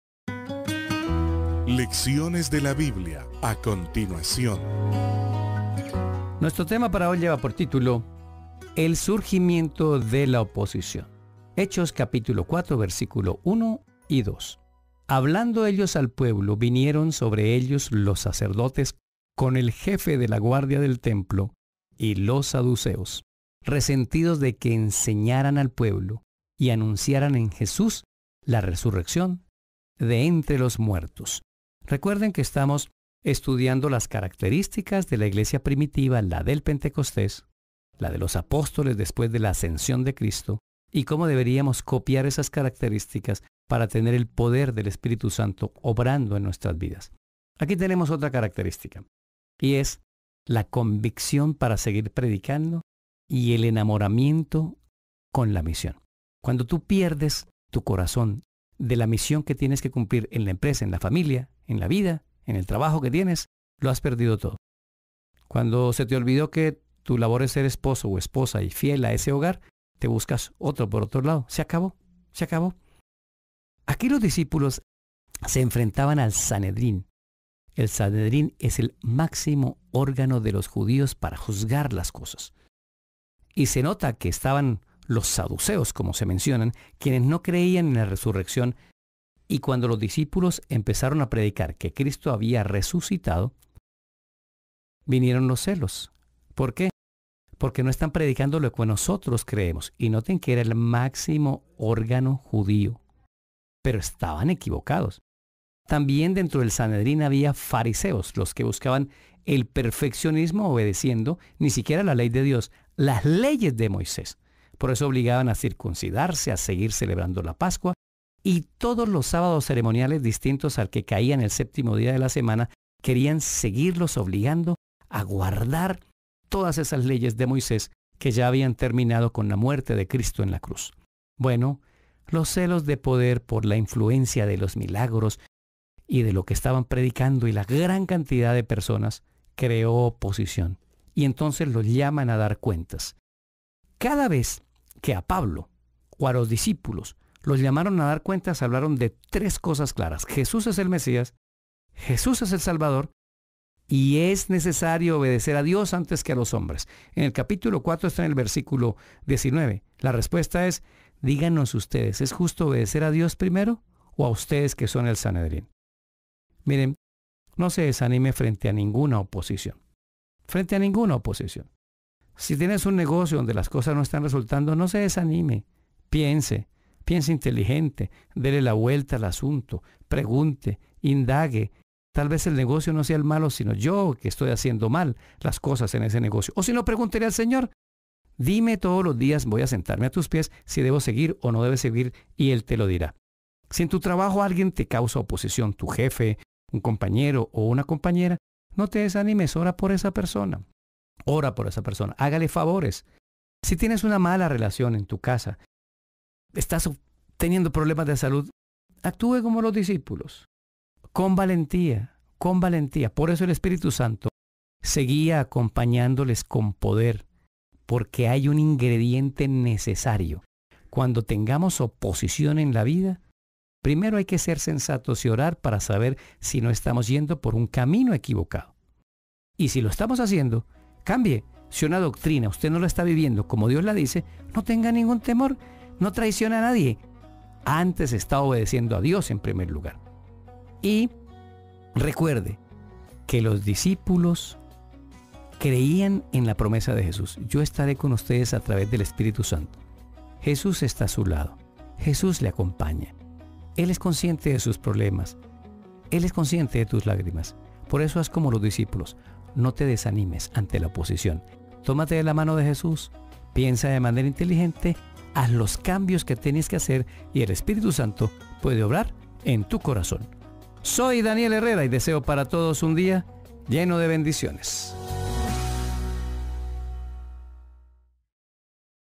El surgimiento de la oposición
Hechosn4:1-2 Hablando ellos al pueblo, vinieron sobre ellos los sacerdotes con el jefe de la guardia del templo, y los saduceos, resentidos de que enseñasen al pueblo, y anunciasen en Jesús la resurrección de entre los muertos.